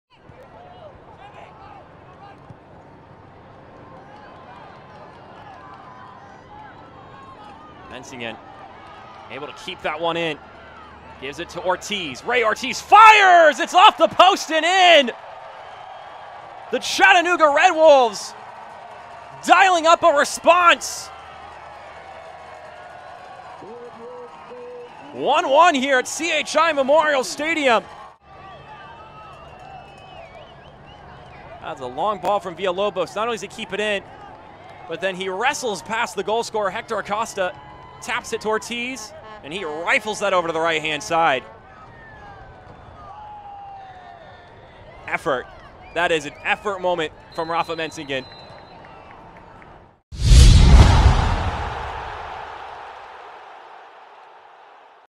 Rey Ortiz with a Goal vs. Charlotte Independence
Rey Ortiz (Chattanooga Red Wolves SC) with a Goal vs. Charlotte Independence, 06/22/2022